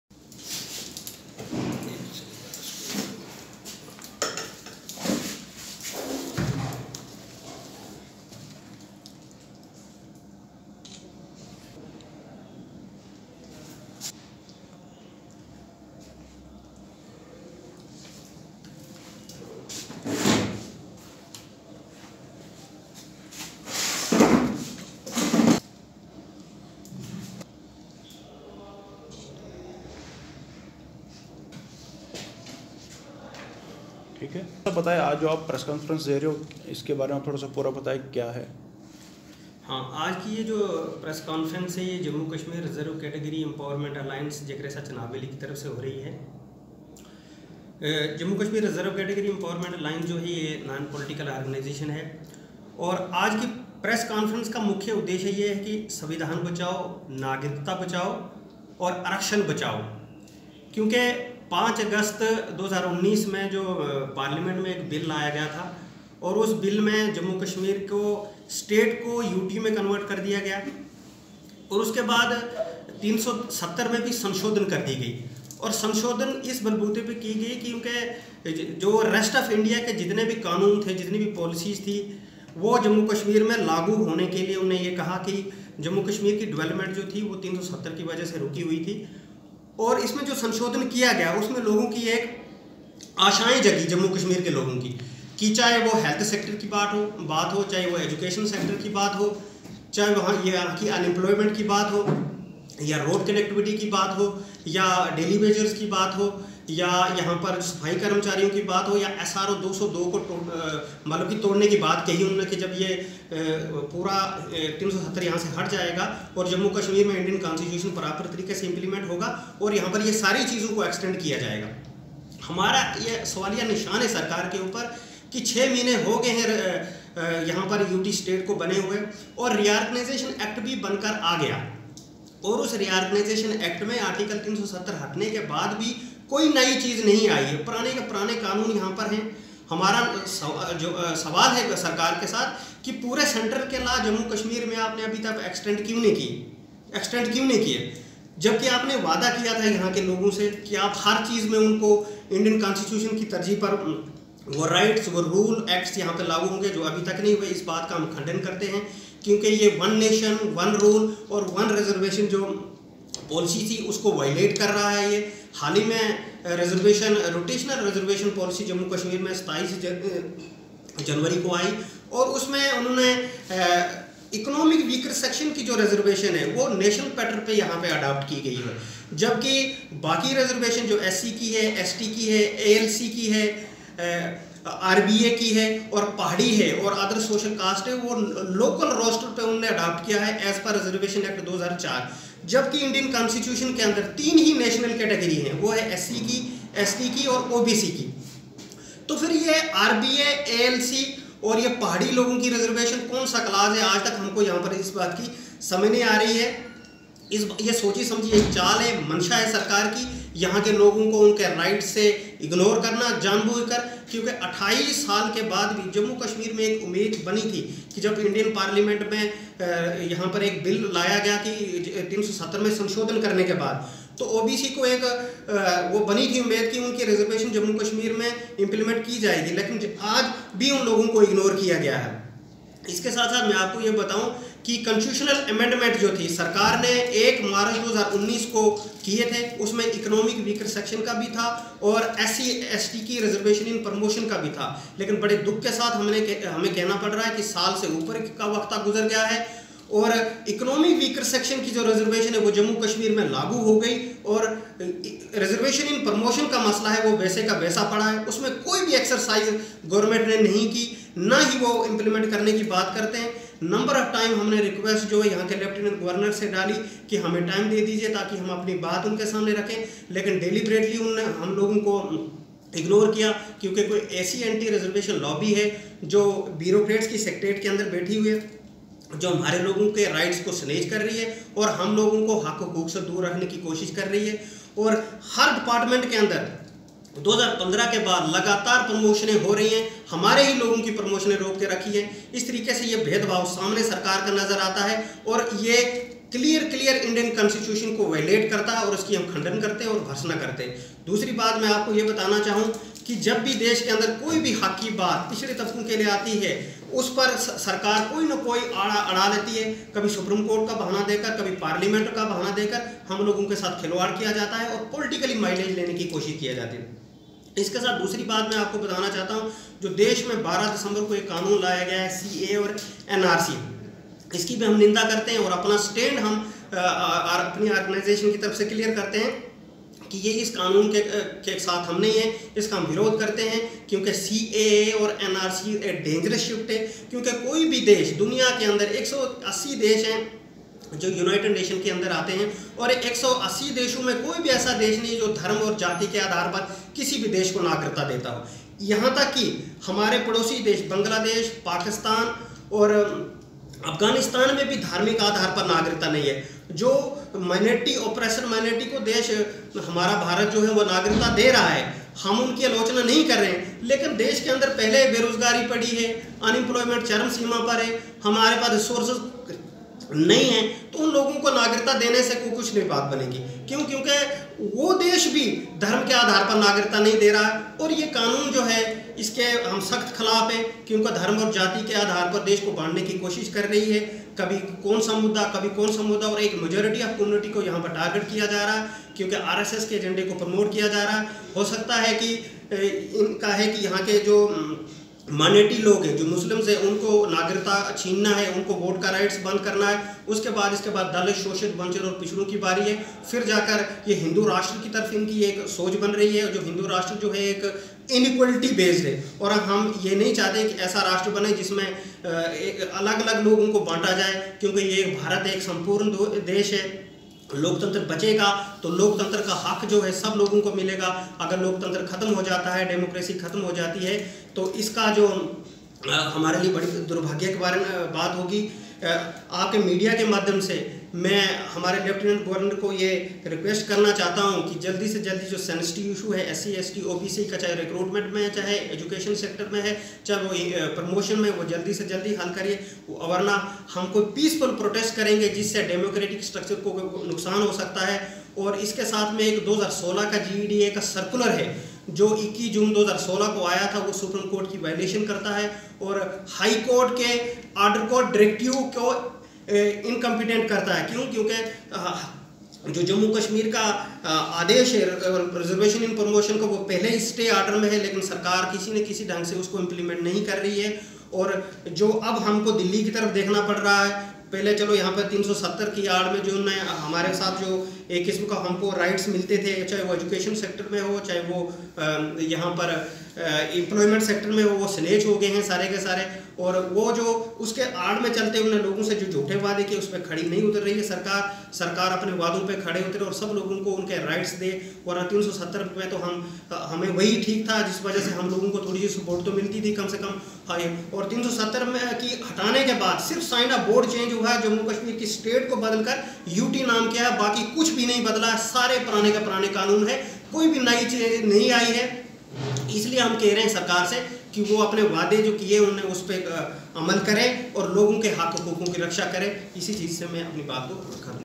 Jammu Kashmir Reserve Category Empowerment Alliance Press Conference Doda
Jammu kashmir Reserve Category Empowerment Alliance Press Conference Regarding Resarvation In promotion Chenab Valley